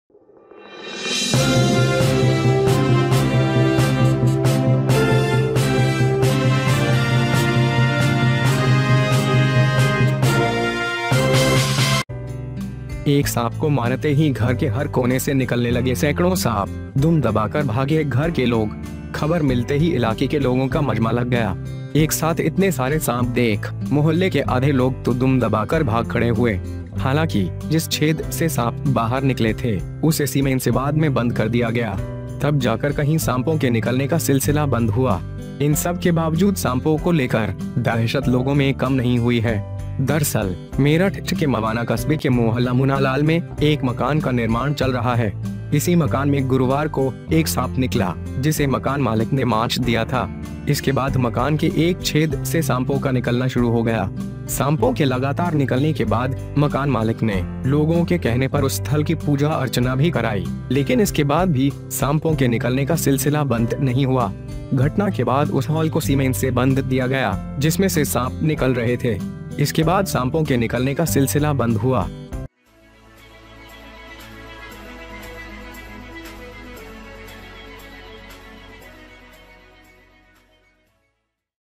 एक सांप को मारते ही घर के हर कोने से निकलने लगे सैकड़ों साप... दुम दबाकर भागे घर के लोग
After killing a snake, hundreds of snakes started running from every corner of the house.
As soon as the news was received, the people of the area were subjected to penance. Seeing so many snakes together, half of the people in the neighborhood stood by their feet. However, the hole from which the snake came out was closed later from the cement. Then there was a close connection between the snakes. Despite all these snakes, terror has not diminished in people.

In fact, a house is under construction in Mohalla Munnalal of Mawana town of Meerut. On Thursday, a snake came out, which was killed by the landlord. After this, snakes started to emerge from a hole in the house. After the continuation of the snakes, the landlord also offered worship of the place on the people's request, but even after this, the snake out of the snake was not closed. After the incident, the hole was stopped from the cement, from which the snakes were coming out. After this the snake out of the snake closes.

HJ NEWS !!